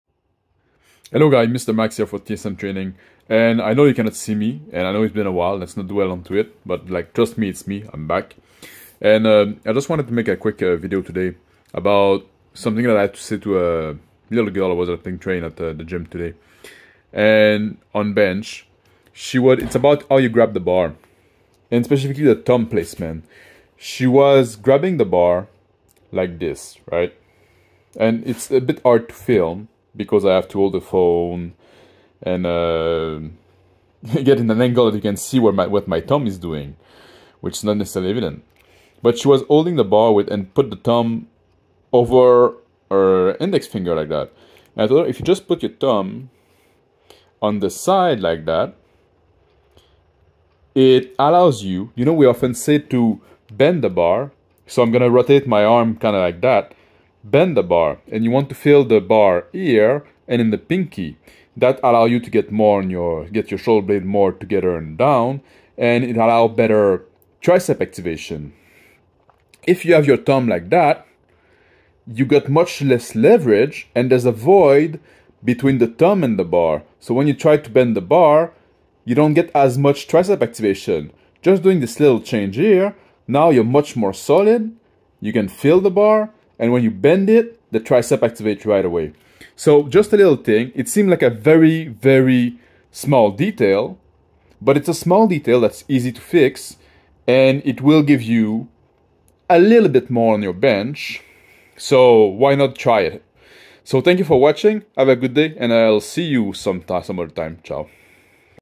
Thumb placement when grabbing the bar for bench
A quick little trick that escapes many people when it comes to how you grab the bar for bench. The way you place your thumb can have an impact on your tricep activation.